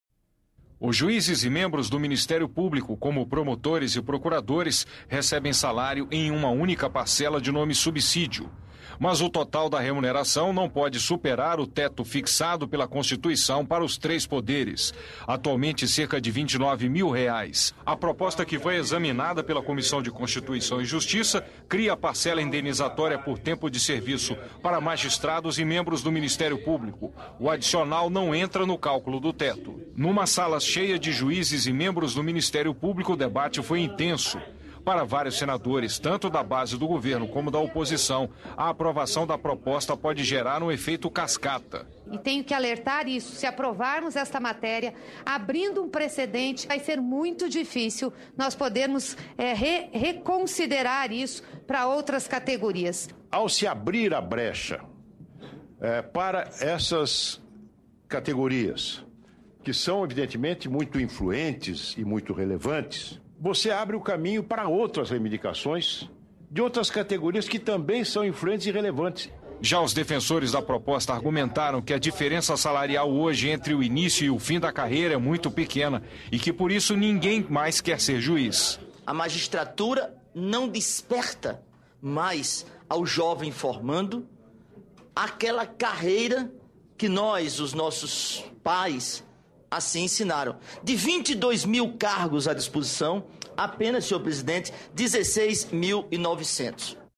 Adicional para magistratura e Ministério Público causa polêmica entre senadores
A aprovação da PEC que cria adicional por tempo de serviço para juízes e membros do Ministério Público provocou debate intenso na Comissão de Constituição e Justiça. O relator, senador Vital do Rêgo (PMDB-PB), diz que a proposta é necessária para incentivar a carreira da magistratura. Mas parlamentares do governo e da oposição, como o líder do PSDB, senador Aloysio Nunes (SP), afirmam que a PEC pode gerar efeito cascata para outras categorias do funcionalismo público. Veja a reportagem de Antonio Machado, da TV Senado.